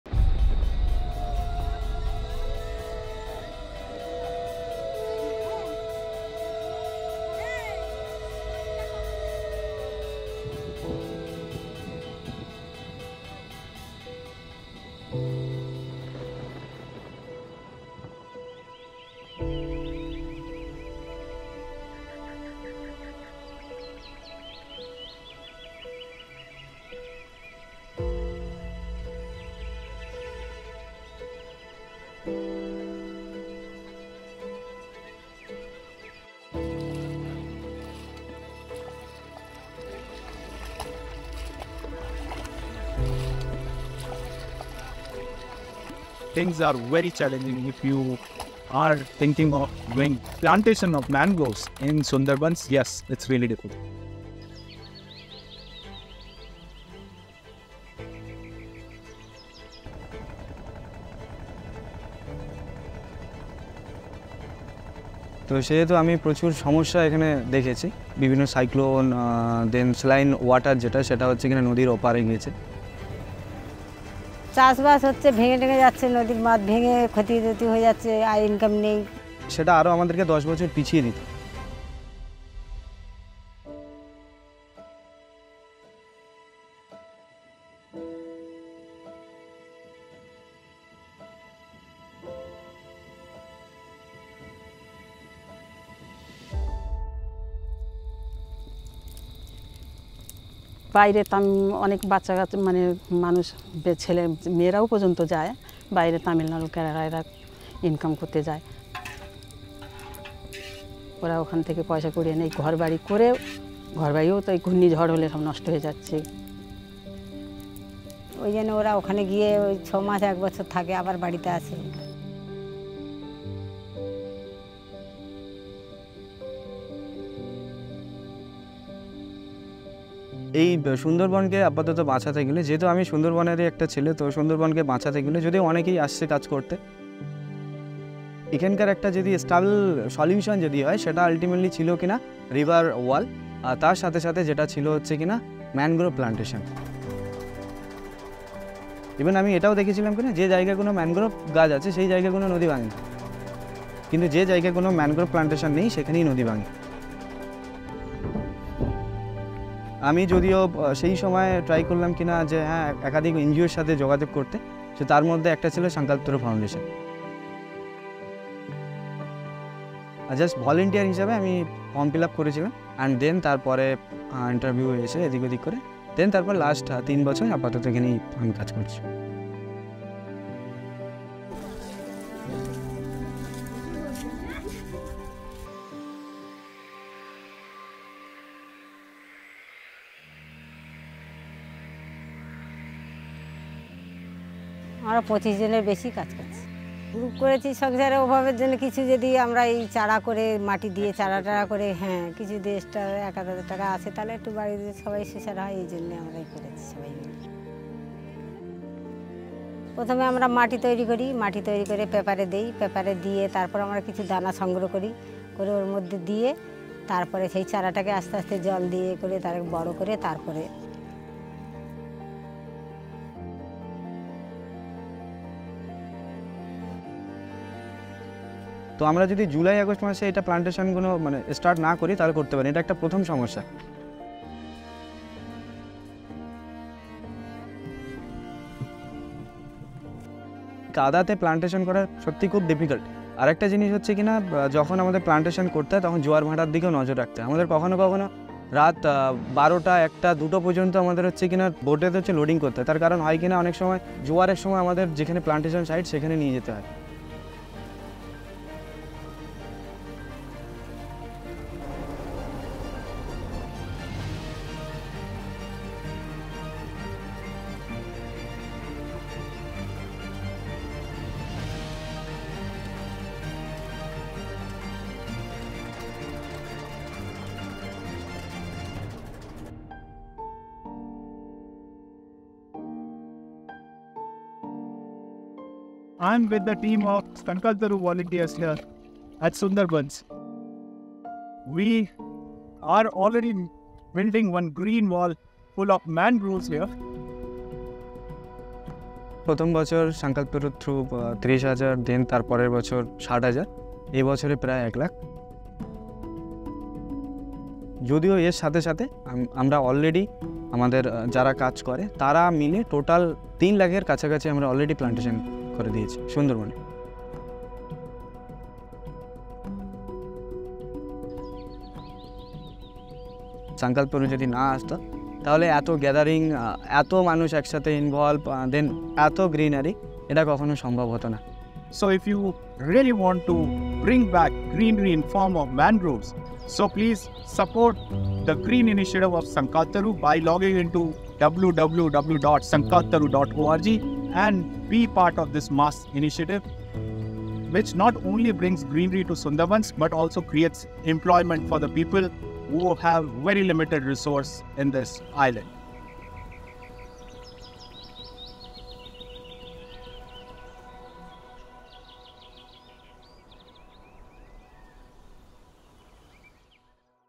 A Green Mangrove Wall to Protect Sundarbans
The Sundarbans, a sprawling mangrove forest across West Bengal in India, stands as a critical and unique ecosystem that holds immense importance for the environment and local communities. This intricate mangrove ecosystem, which is a biodiversity hotspot and a natural carbon sink, is under threat due to rising sea levels and frequent cyclones. Growing and maintaining mangroves is a challenging task that requires collaborative efforts and community involvement.

In this video, watch our journey centered on safeguarding this delicate ecosystem in Sunderbans, transforming this mission from seeming 'impossible' into a resounding 'possible’. This initiative not only elevated the social and economic status of rural women but also served as a protective shield to the local communities against natural disasters. We are fully committed to constructing the Green Wall of Mangroves, and your support is critical to our mission.

SankalpTaru (Youtube) -